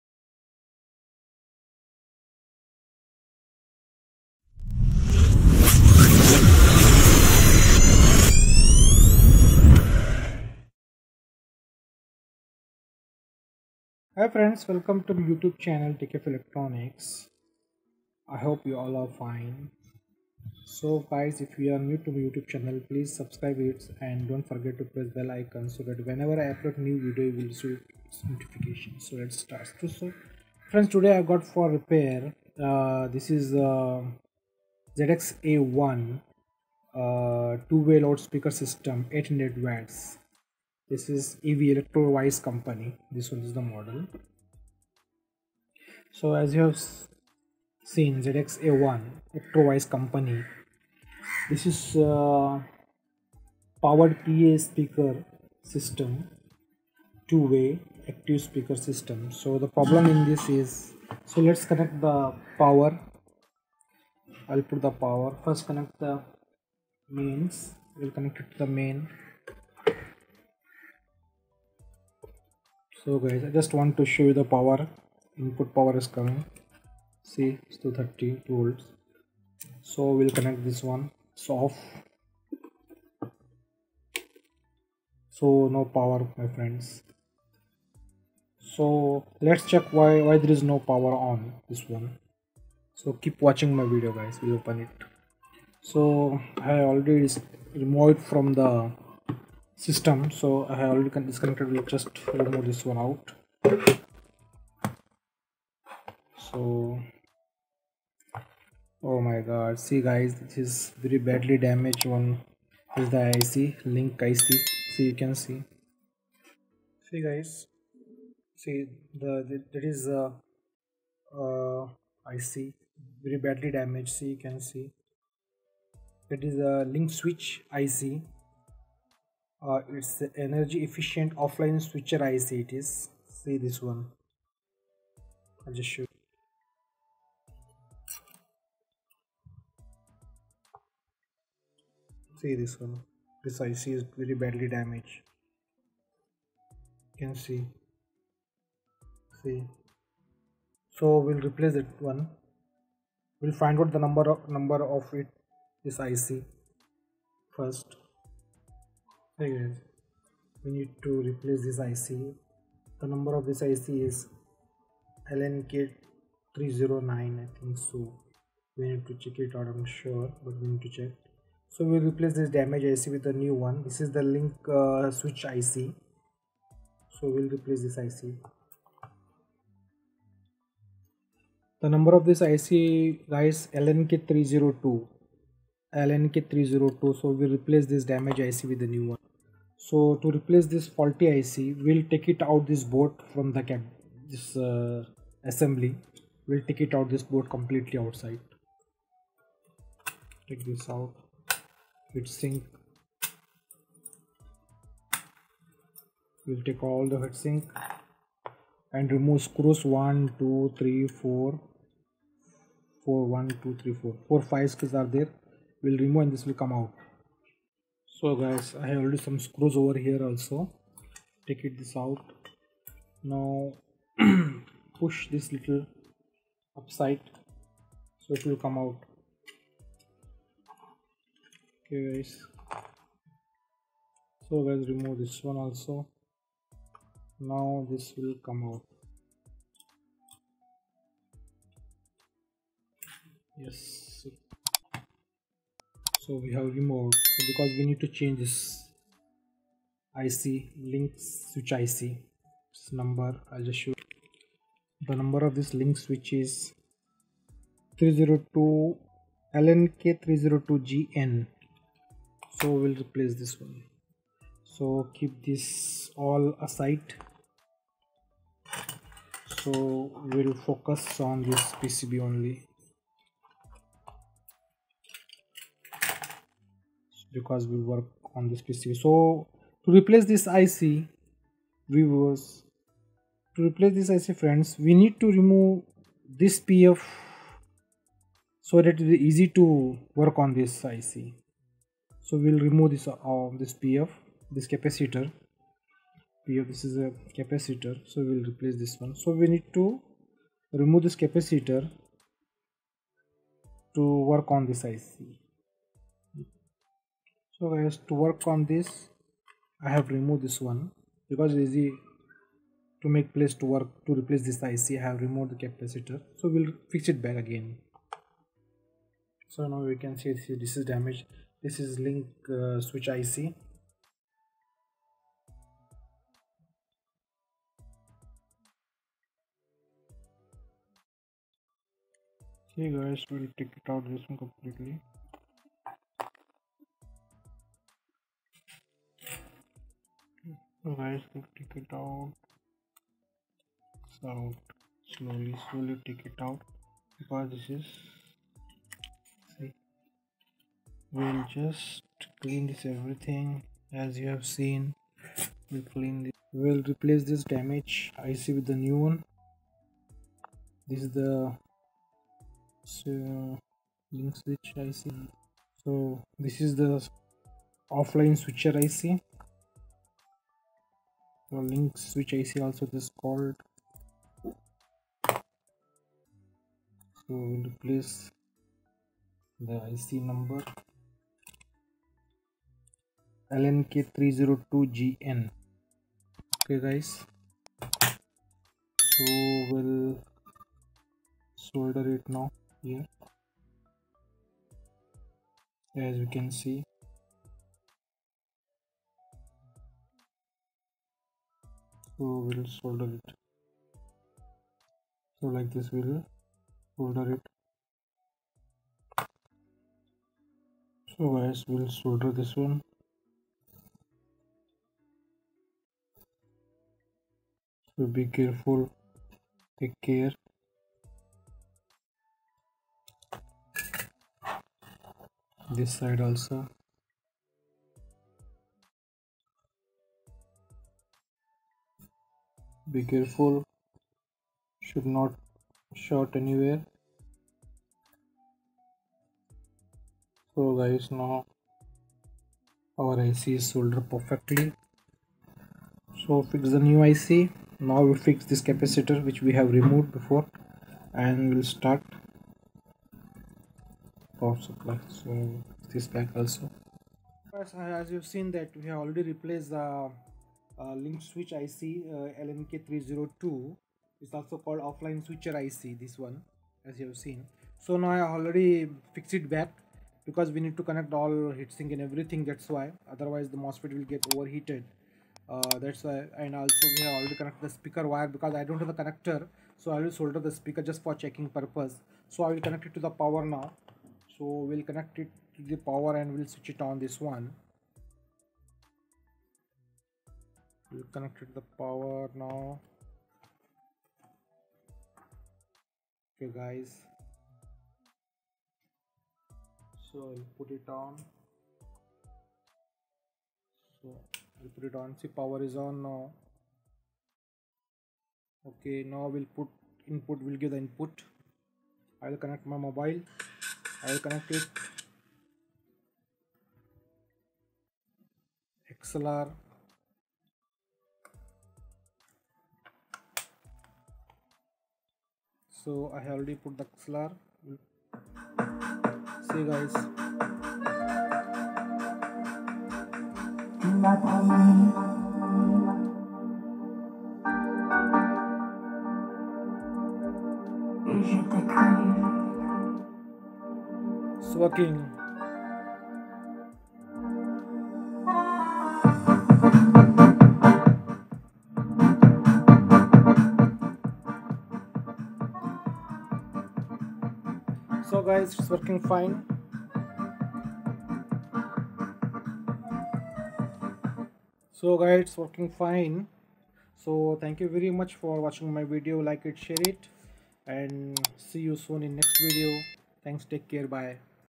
How to Repair Dead Electro-Voice (Ev) model ZXA1-90 Amp. Module
In this video I have shared How to Repair Dead Electro-Voice (Ev) model ZXA1-90 Amp. Module

Soon we are going to be 1k family. Thank you everyone !